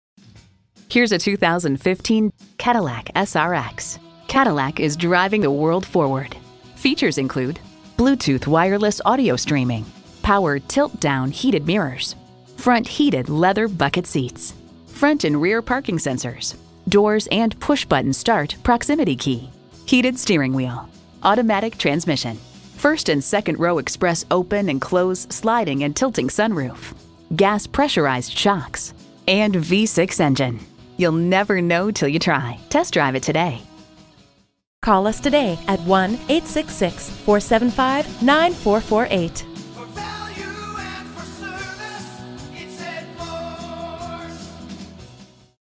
Certified 2015 Cadillac SRX Sunrise FL Miami, FL GW3246 - SOLD
Year: 2015
Make: Cadillac
Model: SRX
Trim: Luxury Collection
Engine: 3.6L SIDI DOHC V6 VVT Engine
Transmission: Automatic
Color: Sapphire Blue Metallic
Interior: Shale w/Brownstone accents
Mileage: 37783
Stock #: GW3246
VIN: 3GYFNBE35FS520247

Address:
14401 West Sunrise Bouledvard